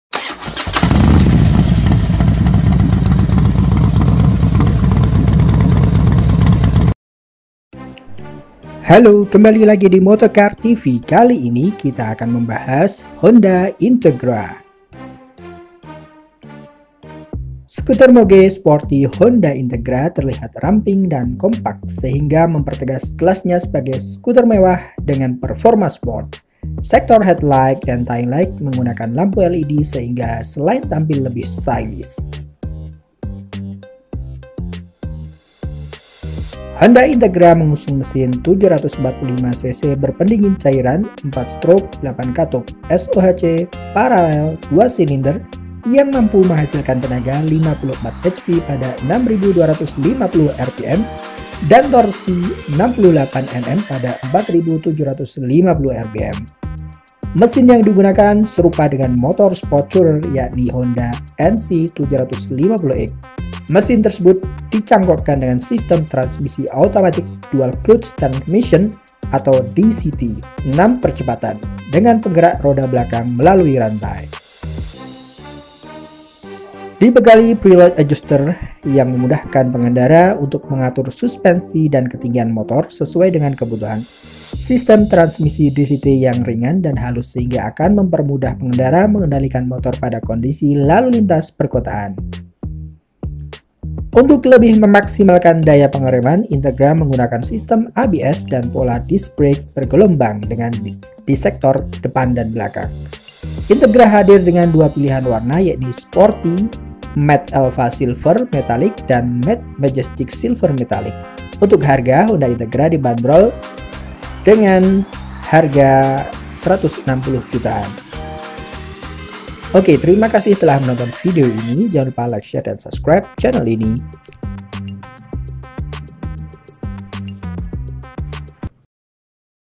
Honda Integra NC750D Big Sporty Scooter | MOTO-CAR TV
The Moge Sporty Honda Integra scooter looks sleek and compact, thereby emphasizing its class as a luxury scooter with sports performance. The headlight and taillight sectors use LED lights so that apart from appearing more stylish.